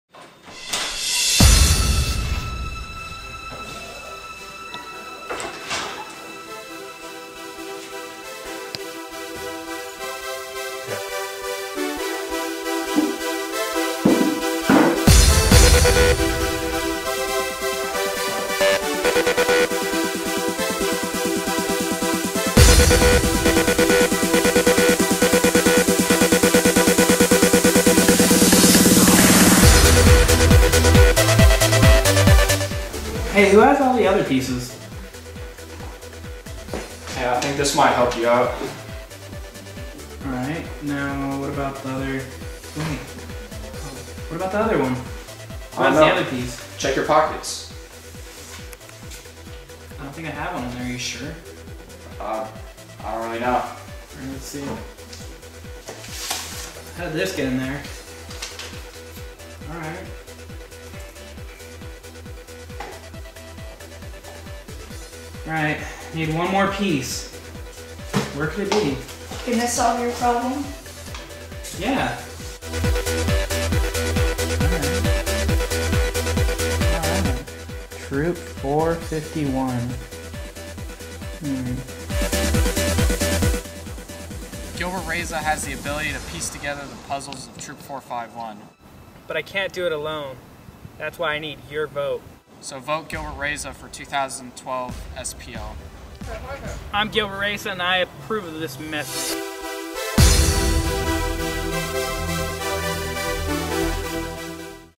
Puzzle Party Promo Video
Promotional video for Gilber Reza of the Puzzle Party for SPL Elections, Winter 2012